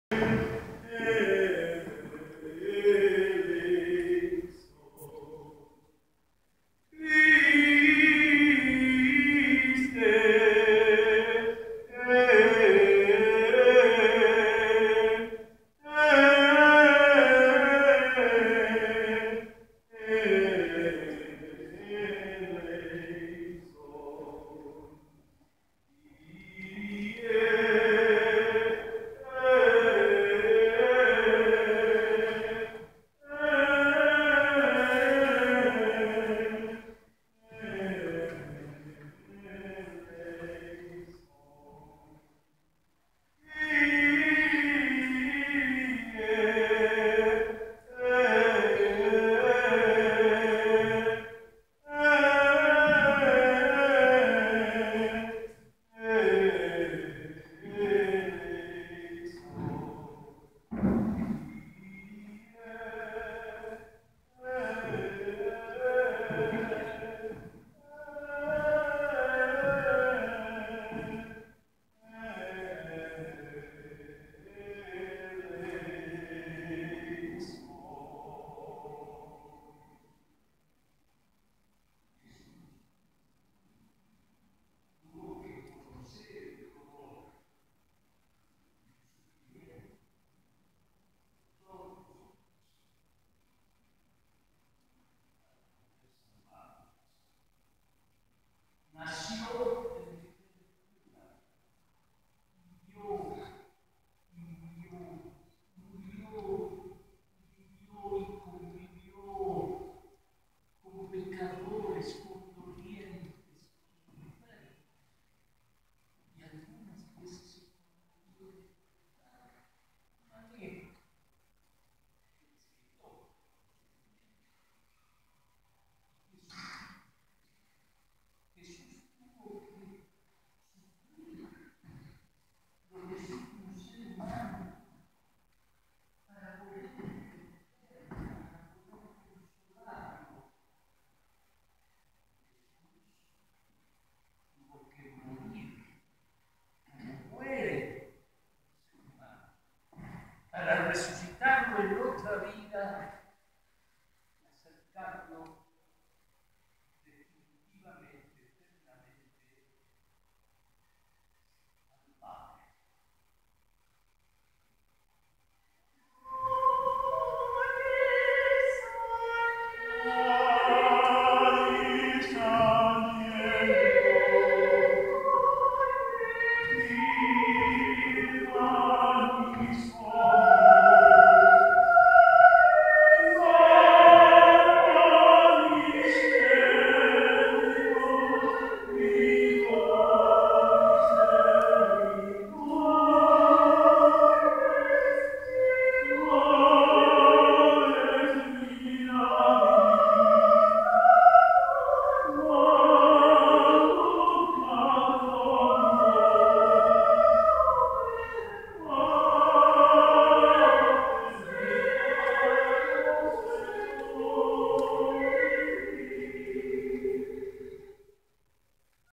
Requiem para Jesús crucificado, ultimos minutos. Noticieros Teatrikando. 4/4
Requiem para Jesús crucificado, tercera parte, Angélica Aragón, Juan Ignacio Aranda, Roberto Damico, Luis Notni. 23 marzo 2016, Museo José Luis Cuevas.